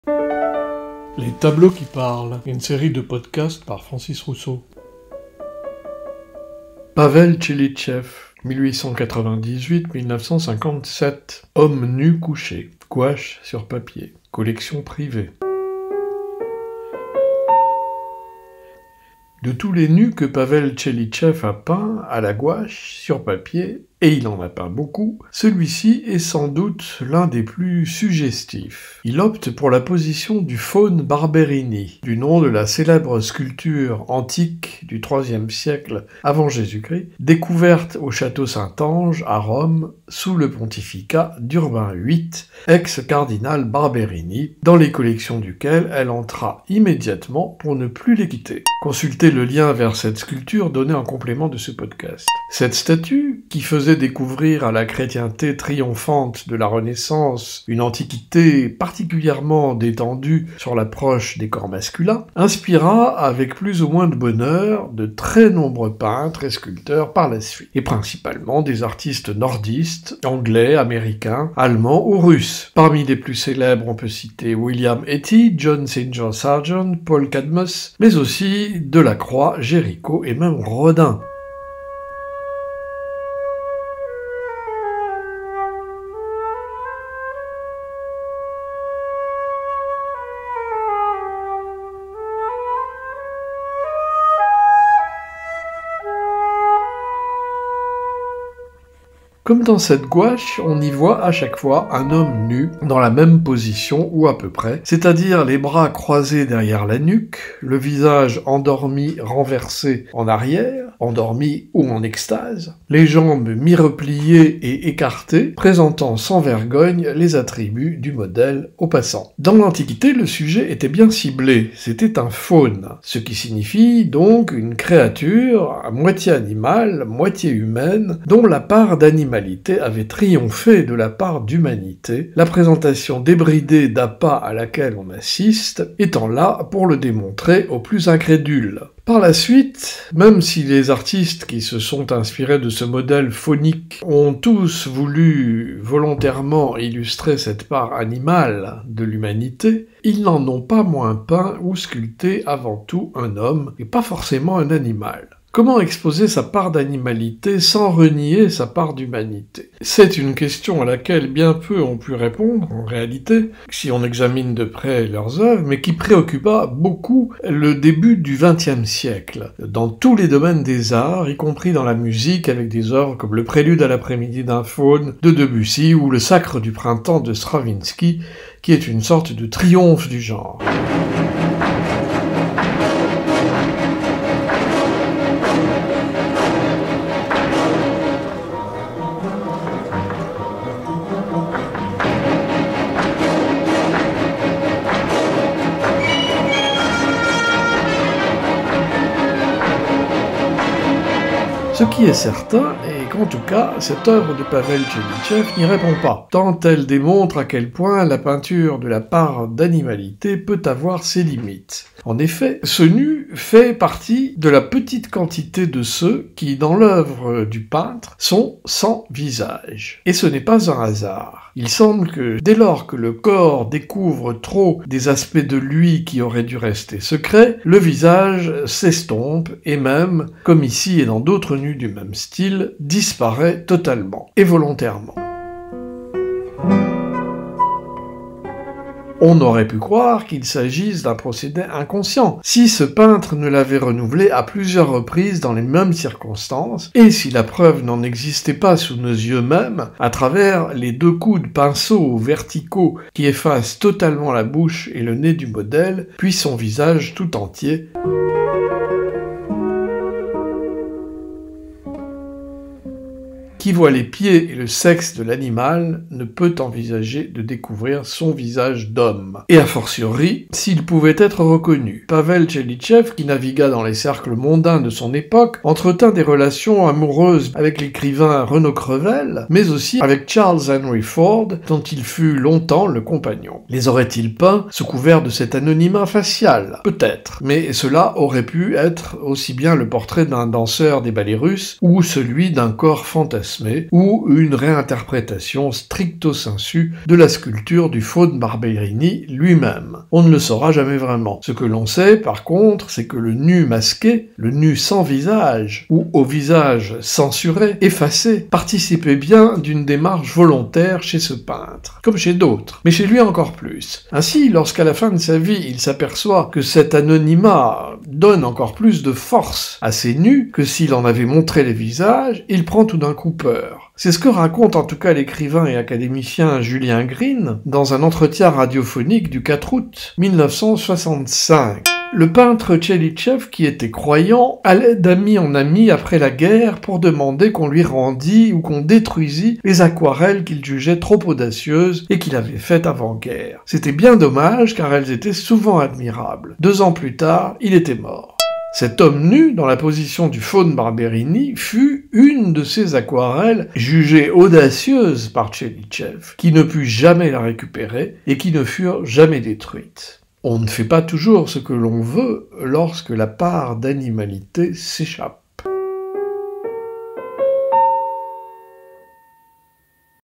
LES TABLEAUX QUI PARLENT N° 83 - De la Nudité sans visage
Podcast culturel, éducatif et ludique pour francophones

De tous les nus que  Pavel Tchelitchev  (ou Tchelitchew) a peint  à la gouache sur papier (et il en a peint beaucoup)  celui ci est sans doute l’un des plus suggestifs. Il opte pour la position dite du Faune Barberini, du nom de la célèbre sculpture antique du 3e siècle avant Jésus Christ découverte au Château Saint Ange a Rome sous le Pontificat d’urbain VIII ex cardinal  Barberini dans les collections duquel elle entra immédiatement pour ne plus les quitter.  (consulter le lien vers cette sculpture  donné en complément de ce podcast)
Cette statue qui faisait découvrir à la chrétienté triomphante de la Renaissance une antiquité particulièrement détendue sur l’approche des corps masculins, inspira - avec plus ou moins de bonheur- de très nombreux peintres et sculpteurs par la suite, et principalement des artistes nordistes, anglais, américains, allemands ou russes.
Pour connaitre la suite, écouter ce podcast.

Lien donnés en compléments à consulter pendant la lecture du podcast :
- Le Faune Barberini